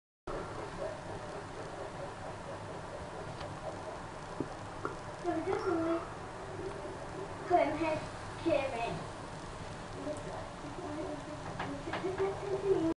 Fly cleaning itself
A small fly, possibly a Lonchopterid, cleaning itself in the bathroom. Taken on my Canon IXUS 265